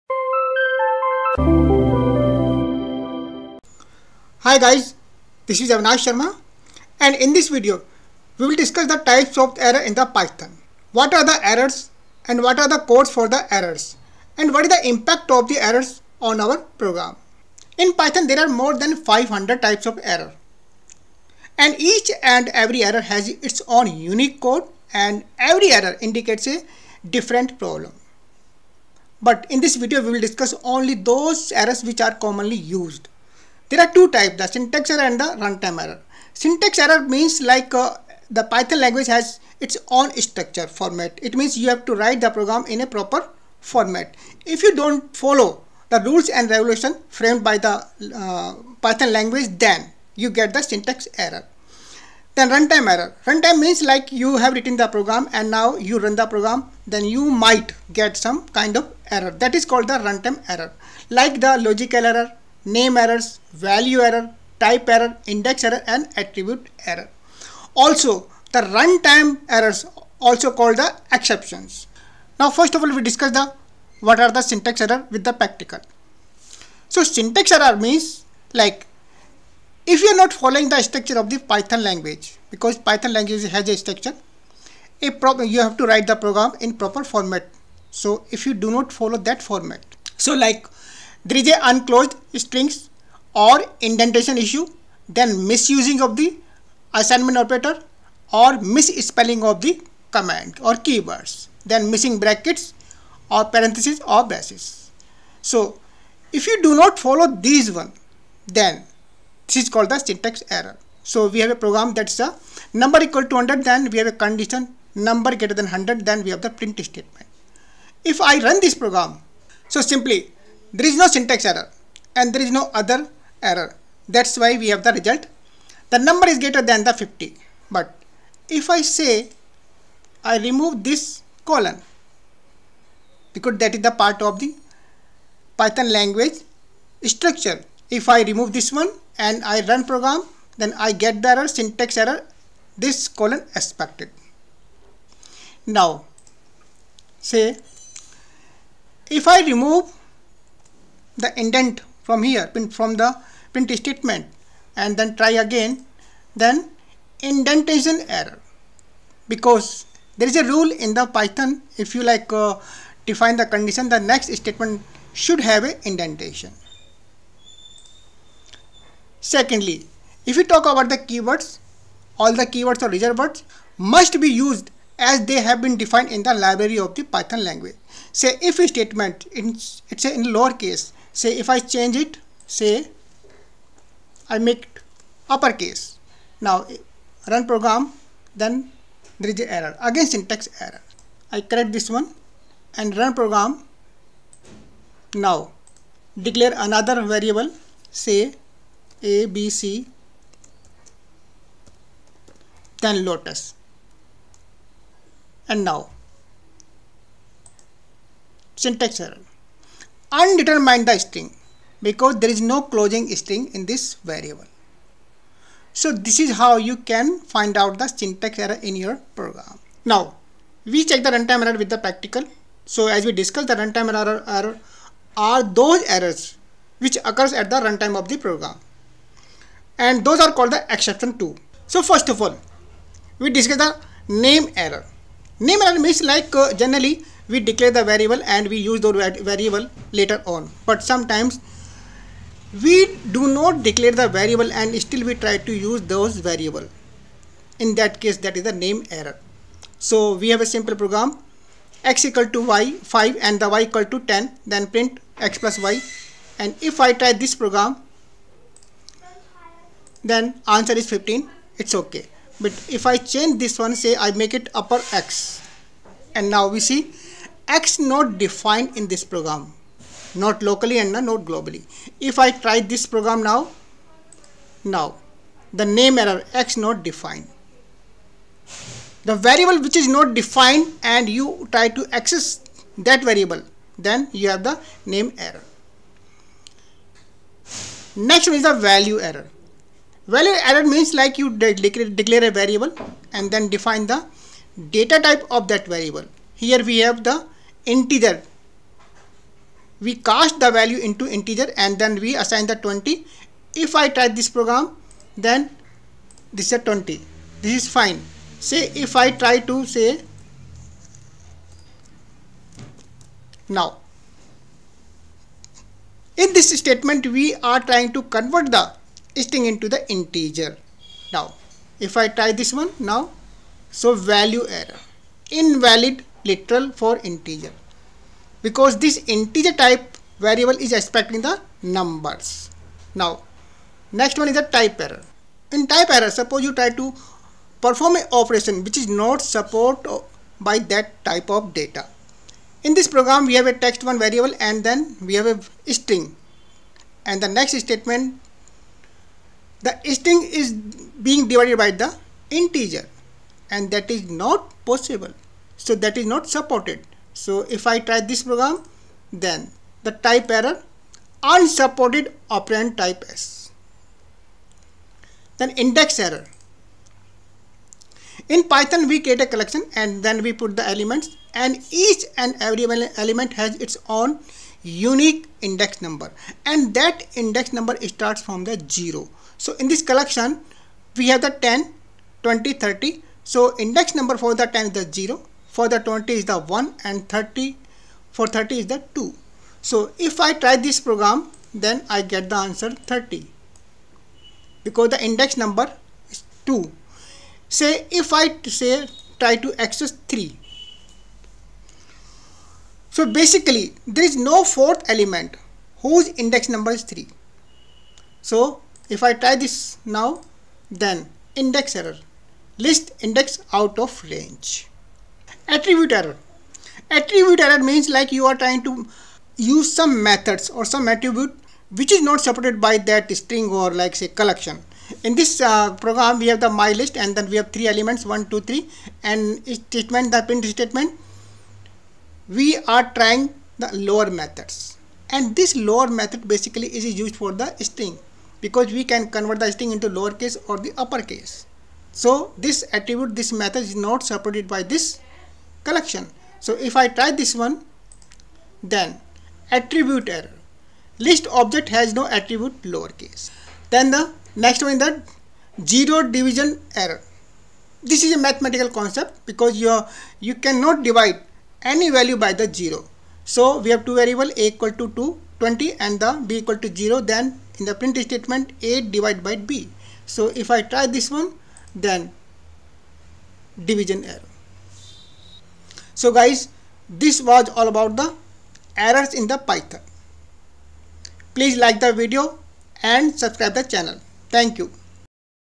Types of Errors and Exceptions in Python
In this video learn what are types of errors in python